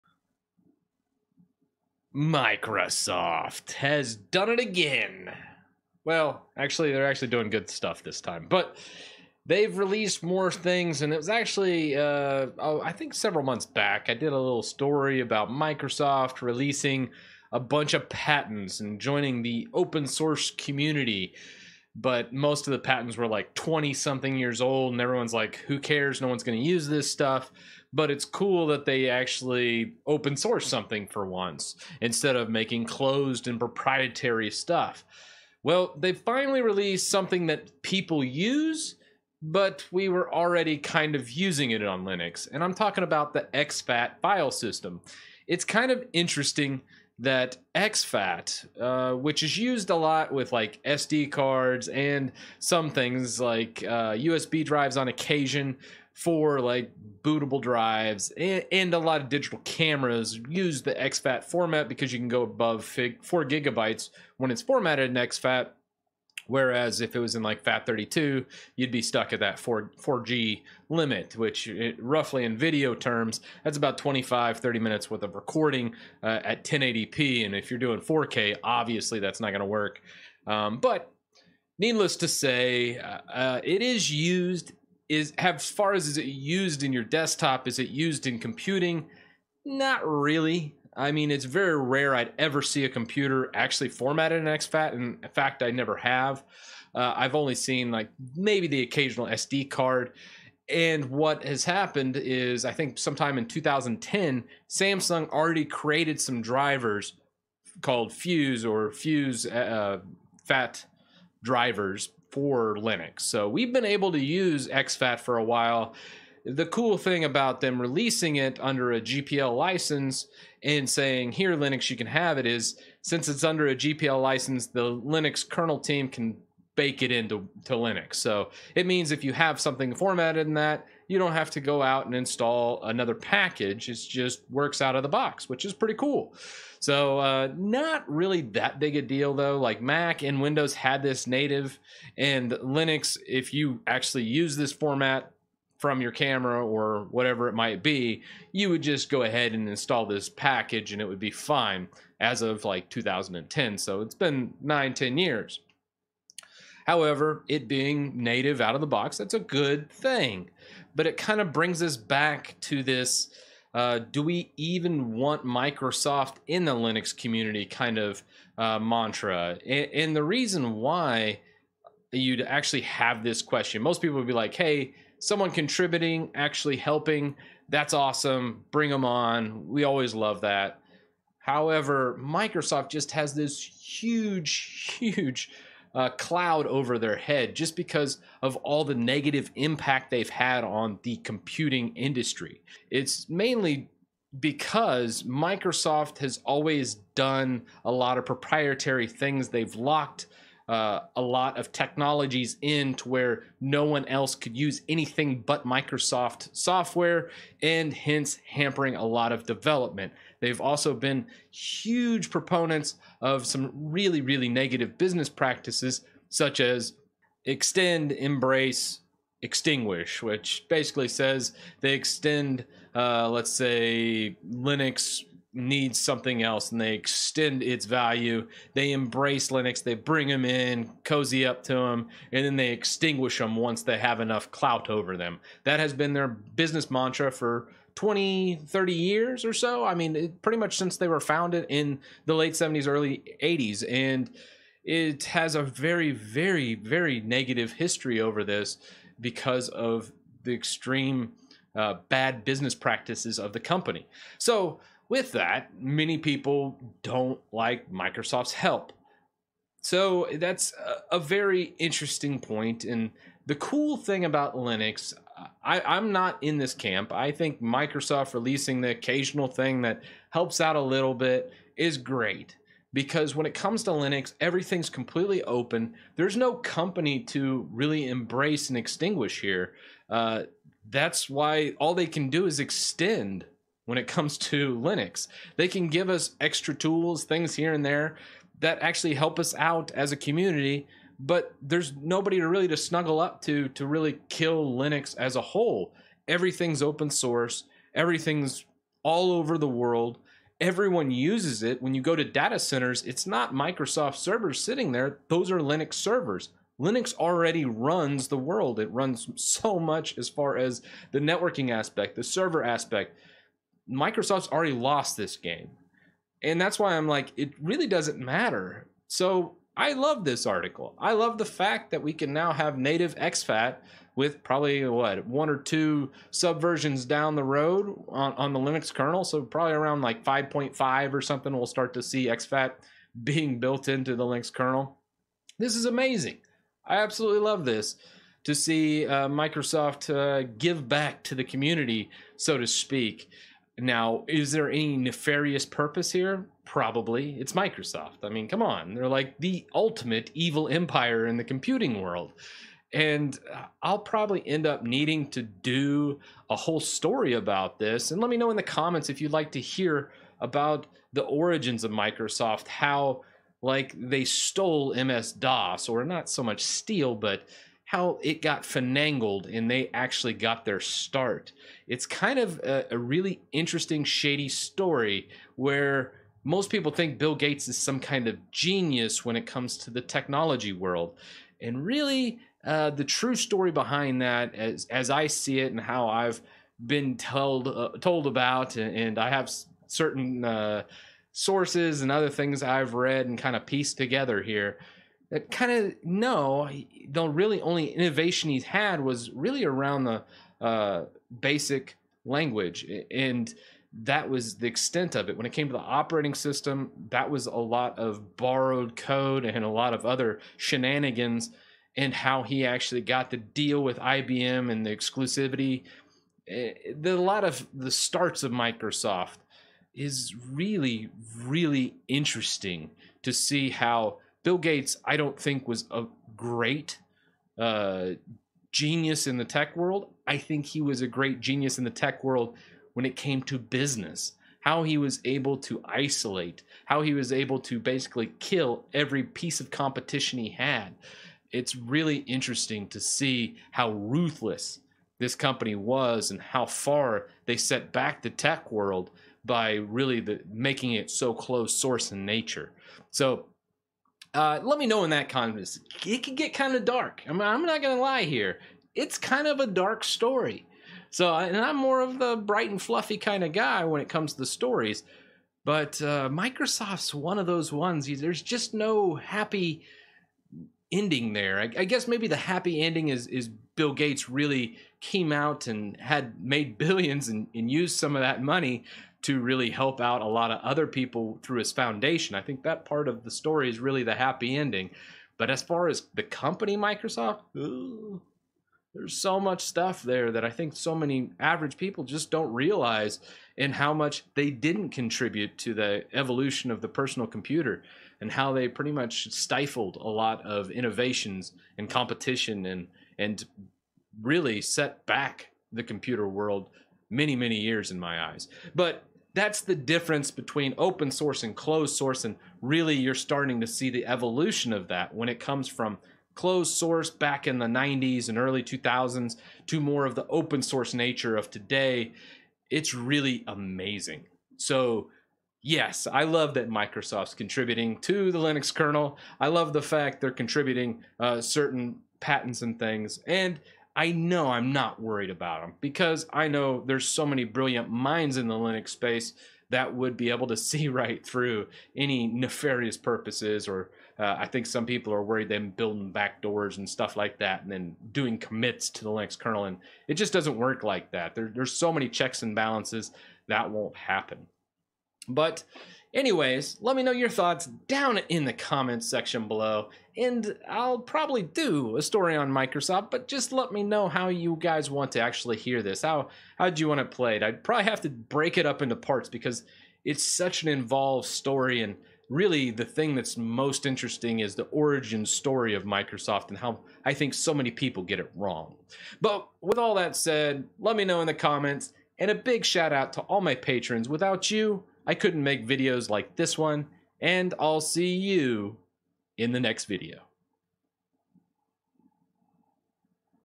ExFAT Linux | Microsoft is "Extending" Linux
The Evil Empire Resurfaces! This time they have contributed ExFAT with a full GPL license. Could they be changing their ways? or is there more sinister plans at work.